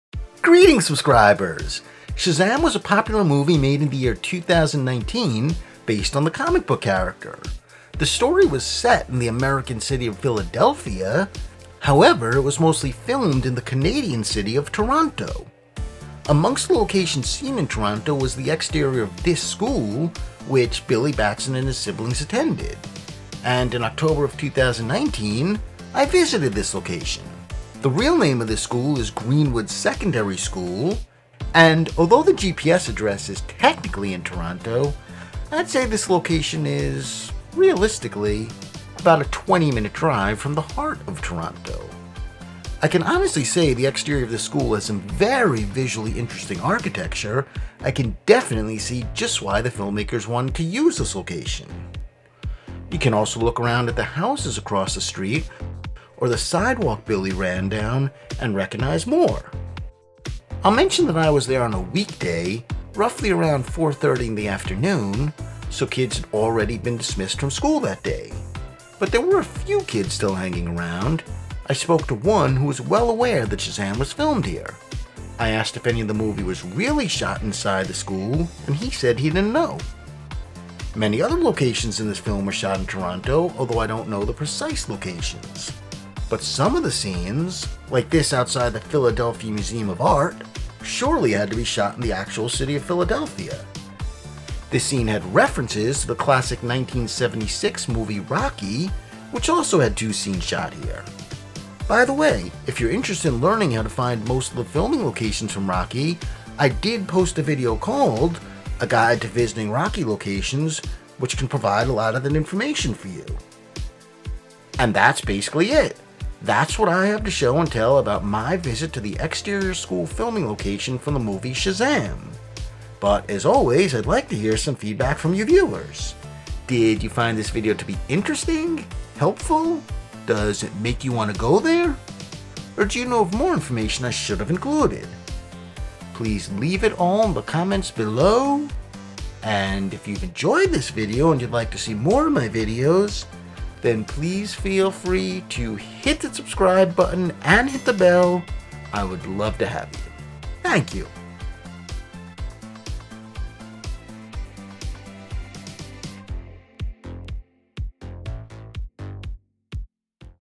Shazam! (2019) School Filming Location
This video is NOT targeted at kids under the age of 13.

Shazam! was a popular movie from 2019 based on the DC comic book character Shazam / Captain Marvel. The movie was directed by David F. Sandberg, and starred Asher Angel as William “Billy” Batson, Zachary Levi as Shazam, Mark Strong as Dr. Thaddeus Sivana, Jack Dylan Grazer as Frederick “Freddy” Freeman, Djimon Hounsou as Wizard, Faithe Herman as Darla Dudley, Grace Fulton as Mary Bromfield, Ian Chen as Eugene Choi, Jovan Armand as Pedro Peña, and John Glover (remember him as Lionel Luthor on Smallville?) as Mr. Sivana. Although the story was set in the American city of Philadelphia, Pennsylvania, the movie was mostly shot in the Canadian city of Toronto. Many scenes were set at Billy Batson’s school. In this video, I show and tell the filming location of that school. I also briefly give some filming location info for the classic 1976 movie Rocky (starring Sylvester Stallone, Talia Shire, Carl Weathers, and Burt Young).